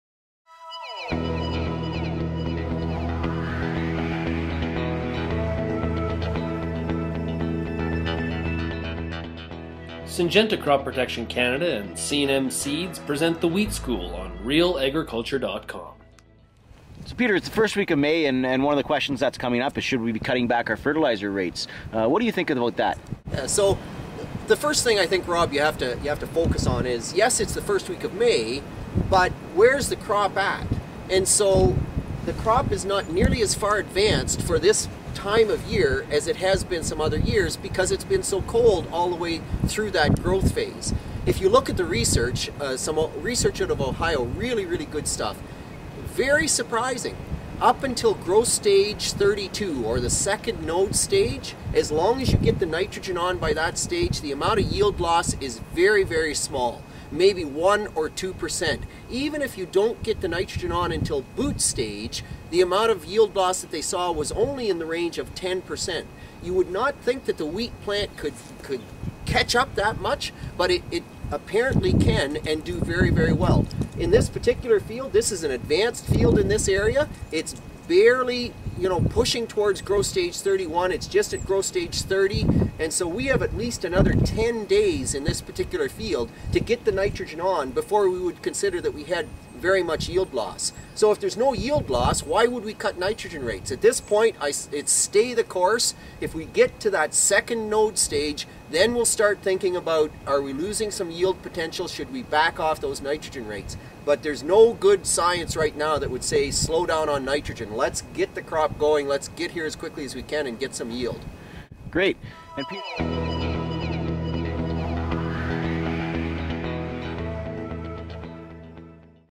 Wheat School - Spring Application of N to your Winter Wheat Crop - Peter Johnson - OMAFRA
RealAgriculture.com talks to OMAFRA Wheat Specialist Peter Johnson about the importance of nitrogen application at the right time.